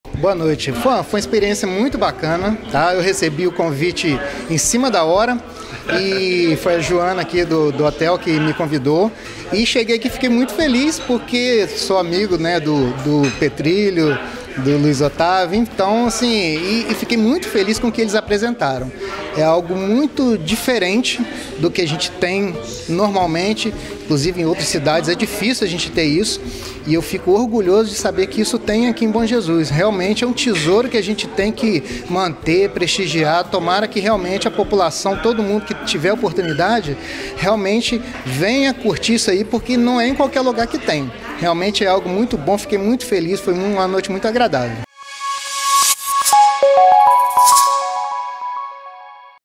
Entrevista com Cássio Pessanha na PRÉ FLICbonjê 2024 - Duo Petrilo Barreto no Hotel São Geraldo
🚨Outros Vídeos Que Você Poderá Gostar🚨